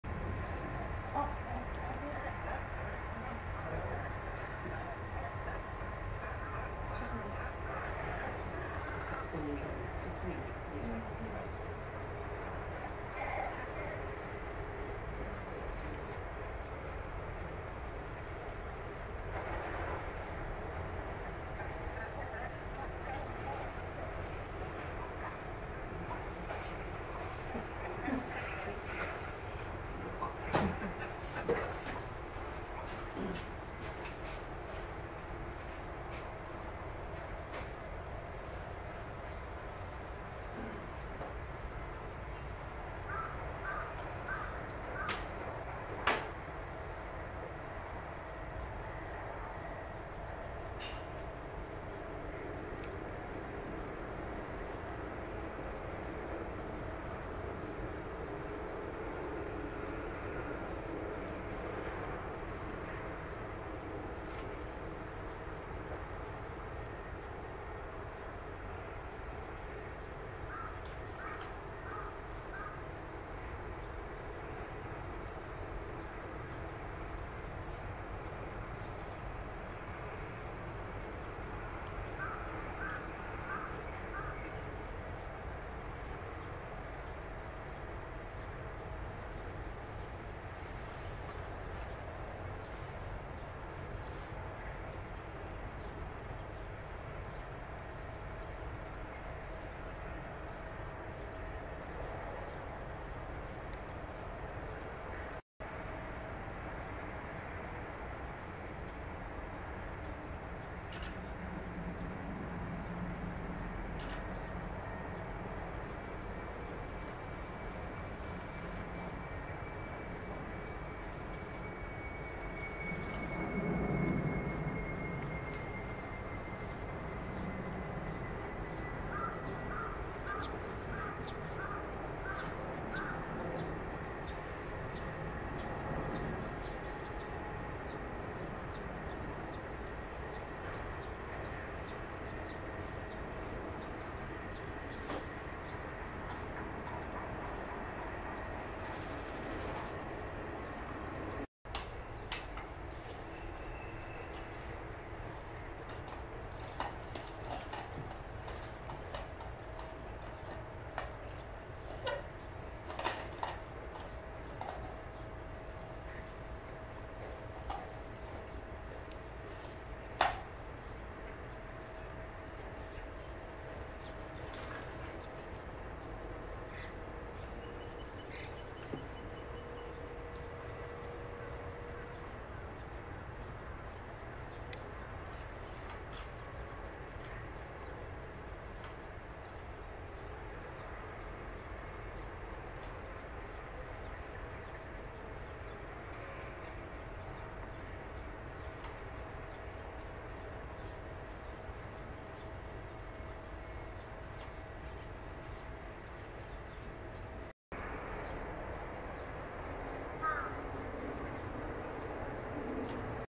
Two cormorants fight for the branch, guess what it wanna do with this branch :p
Thanks for wathcing, hope you enjoy. :D

- I observed for over half an hour, but it finally gave up, lol